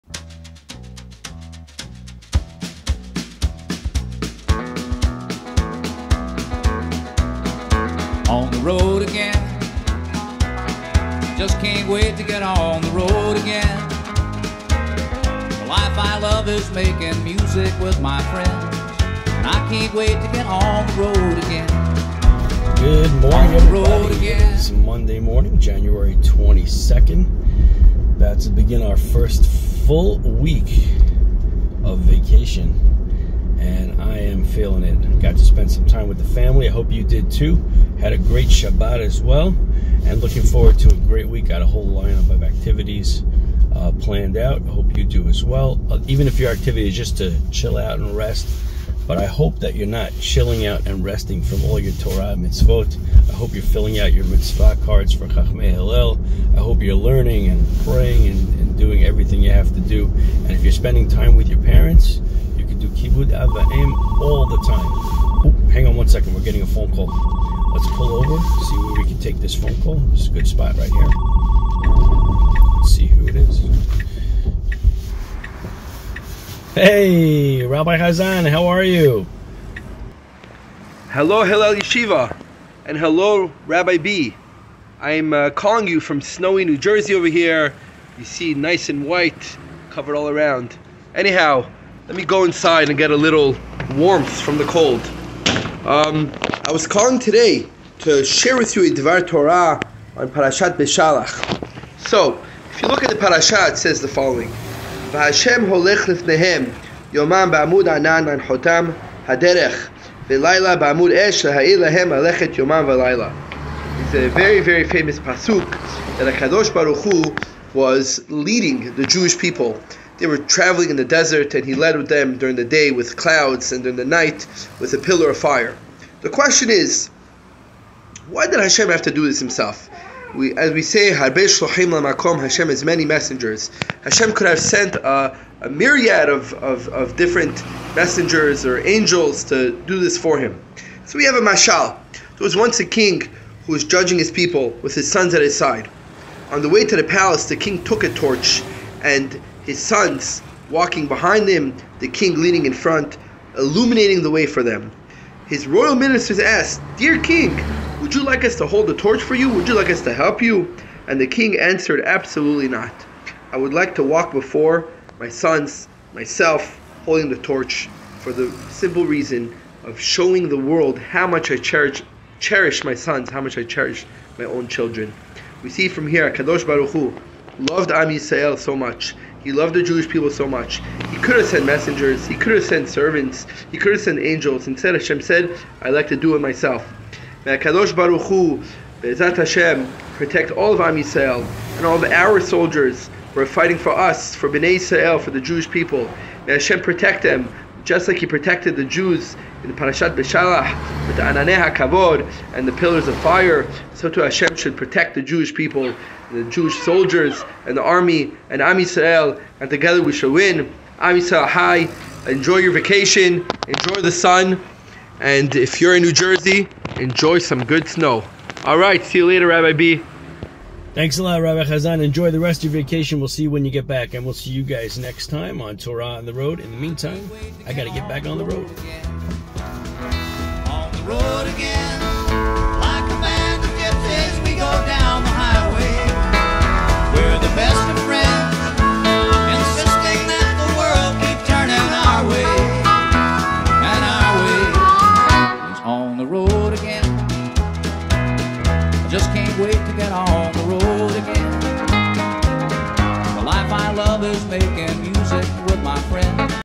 Monday January 22nd 2024
Torah on the Road
Enjoy a daily Devar Torah hosted by Rabbi B
Today's special guest - Rabbi Hazan!!!